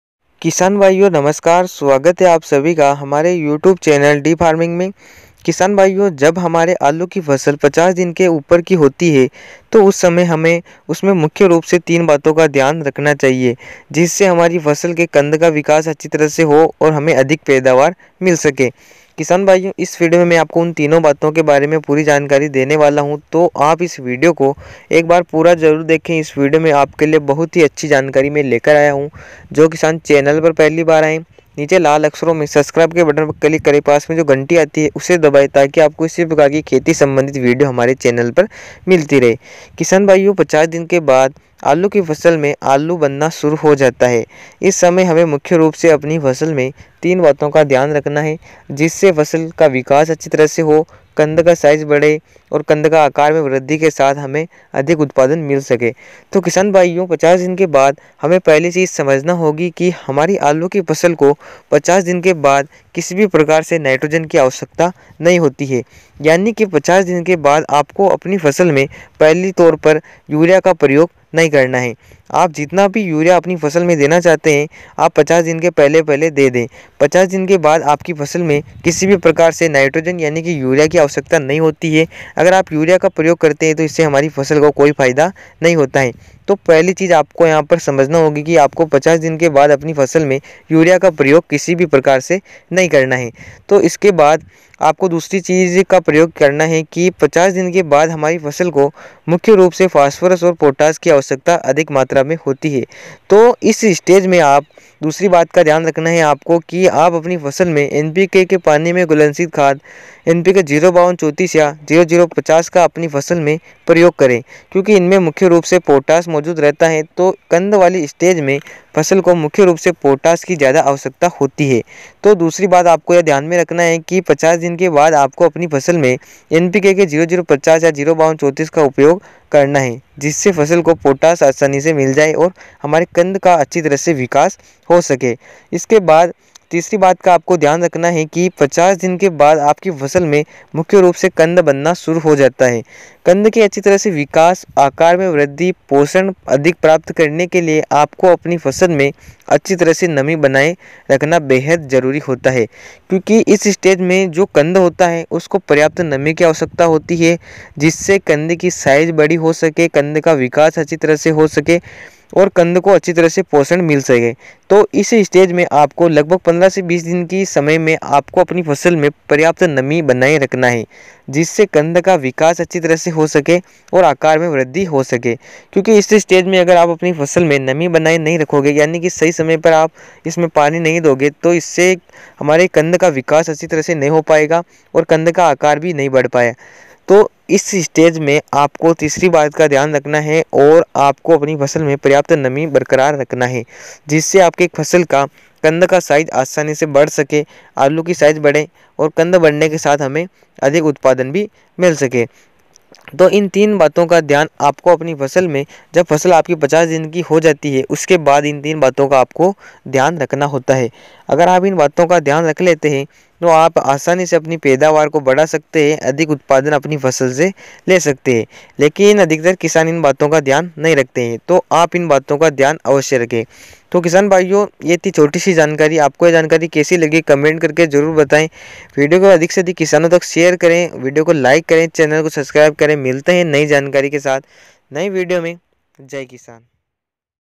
50 दिन के बाद आलू में 3 गलतीयां कभी ना करें//आलू की पैदावार//आलू की खेती//aalu ki pedavar//
इस वीडियो में आपको आलू की पैदावार बडा़ने के बारे में बताया गया है।